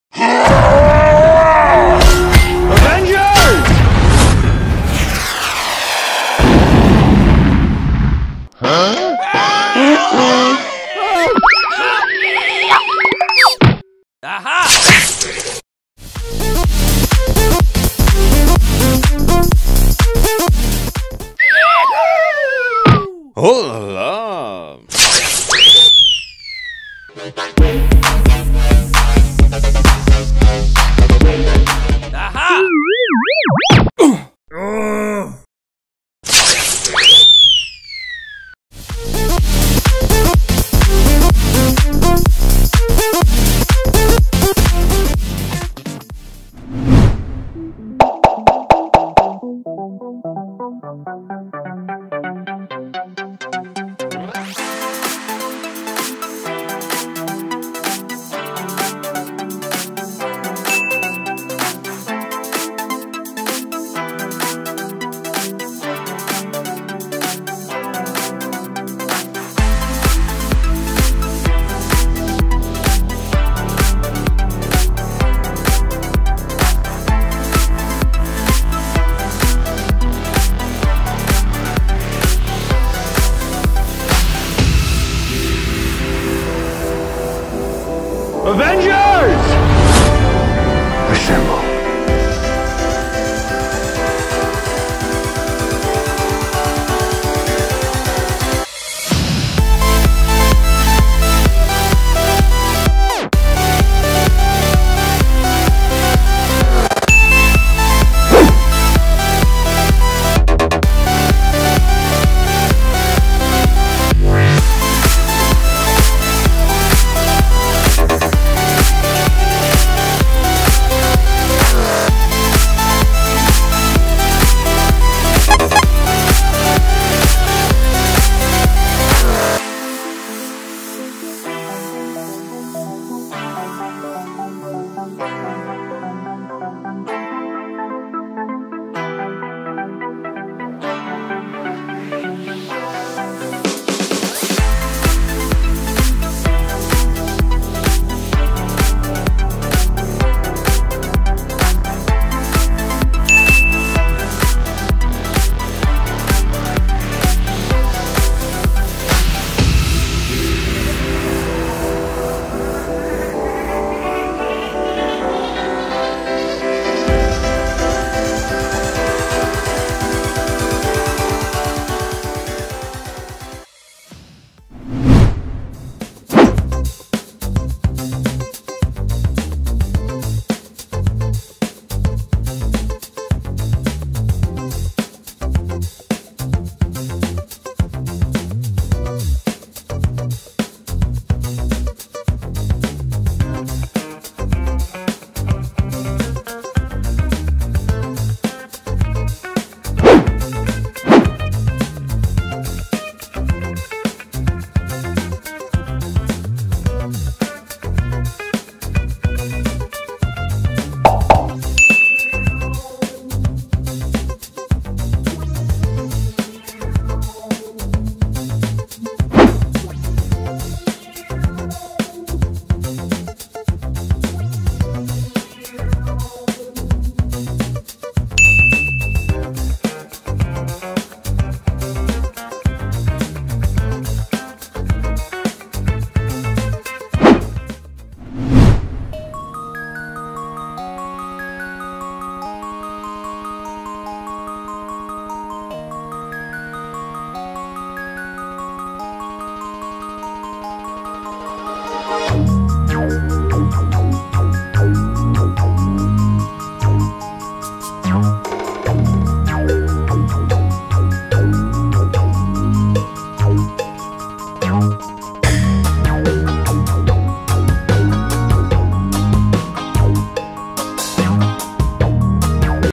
Captain America VS Iron Man with clay 🧟 Superheroes Marvel vs Corona Virus 🧟 Polymer Clay Tutorial
00:00 01. Intro - Review Captain America VS Iron Man
00:47 02. Making Captain America VS Iron Man with clay
03:57 04. Review product with clay
This video shows the process of
 from Sky Clay - Satisfying video Timelapse